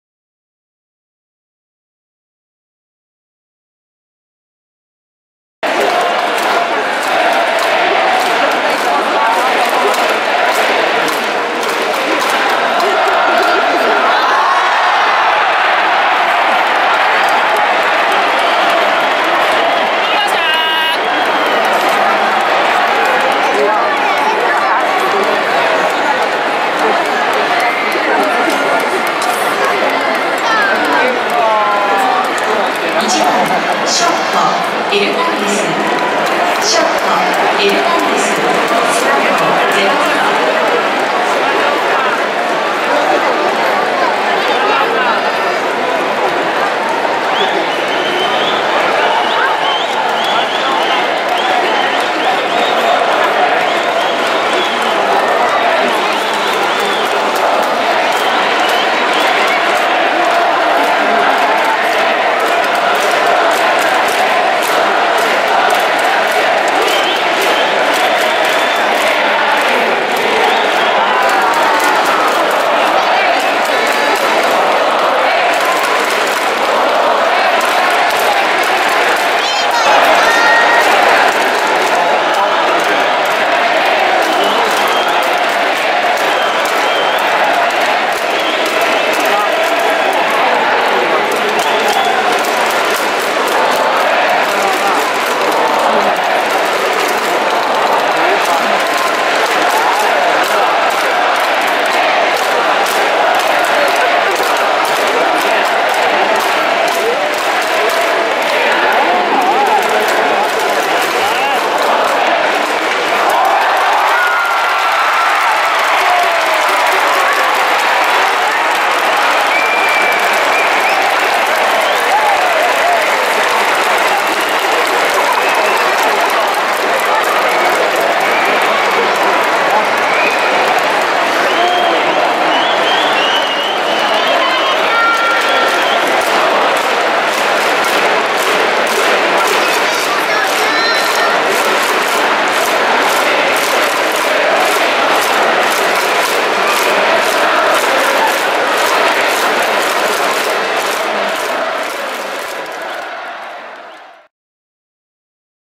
D観２４８ OP戦 新加入 エルナンデスのタイムリーヒット！
2014 3.2 こちらも新加入のエルナンデス内野手　三遊間をやぶるタイムリーヒットでアピール！